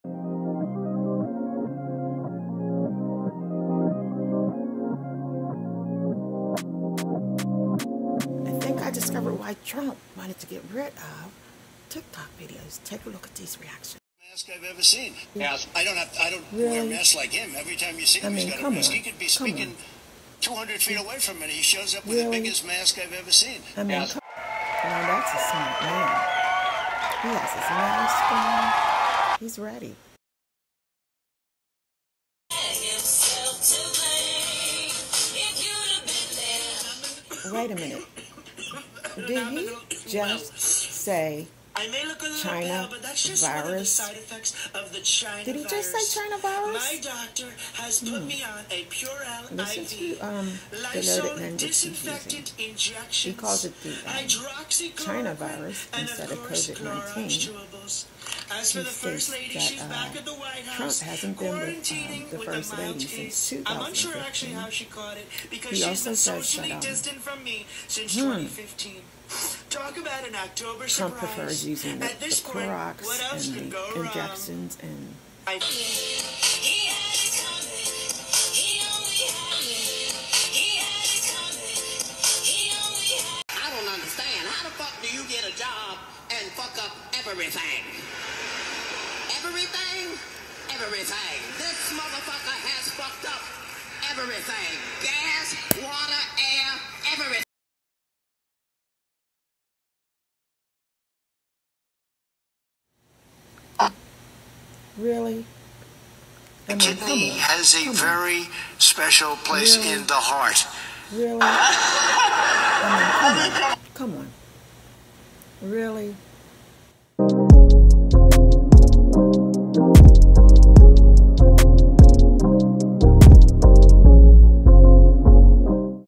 TRUMP'S COV-19 SURPRISE - Tik -TOK reacts and responses
REACTING TO Tik-Tok Trump videos on Tik Tok platform

These are views and opinions based on TiK-Tok public platform that Trump's supports as of 2020.